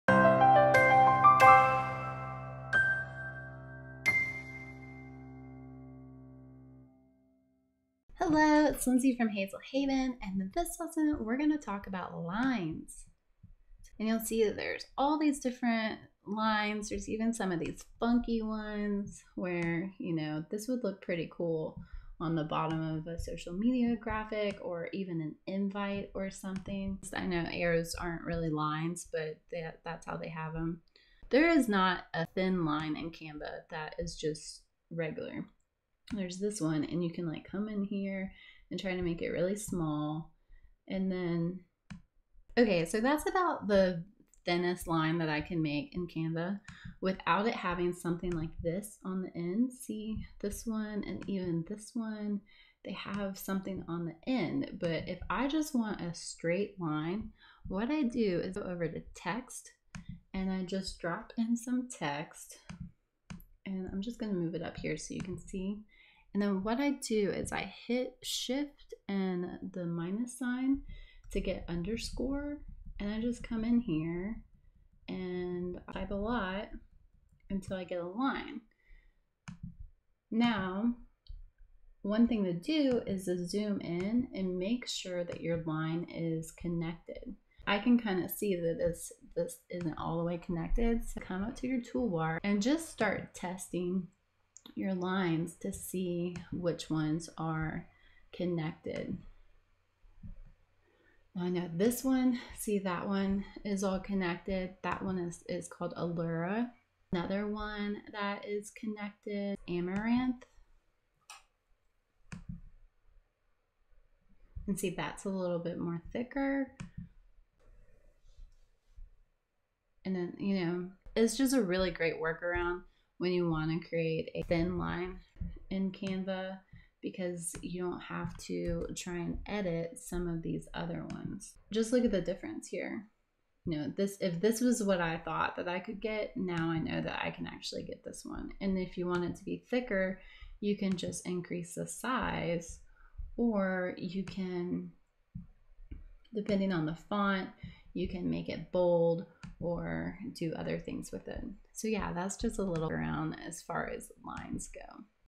How to Create a Thin Line in Canva
In this video you will learn how to Create a Thin Line in Canva:
- All about lines within Canva and How to Create a Thin Line. A simple thin line with Canva.

✂️ This video was edited with Screenflow.

Camera + built in microphone
Budget Friendly Lighting